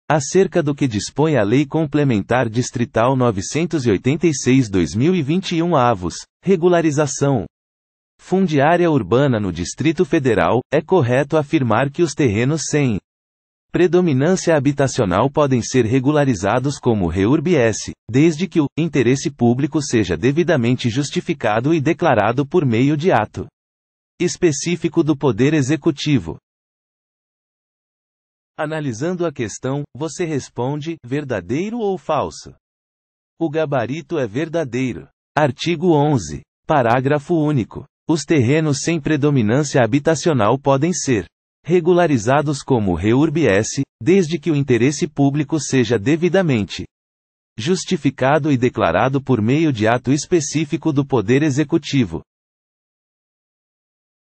QScon Direito: [Art. 11, § único, LC Distrital 986/2021] Texto de Lei
Art. 11, § único, LC Distrital 986/2021 | Texto de Lei

Acerca do que dispõe a Lei Complementar Distrital 986/2021 (Regularização. Fundiária Urbana no Distrito Federal), é correto afirmar que os terrenos sem. predominância habitacional podem ser regularizados como Reurb-S, desde que o. interesse público seja devidamente justificado e declarado por meio de ato. específico do Poder Executivo.

Gabarito: Verdadeiro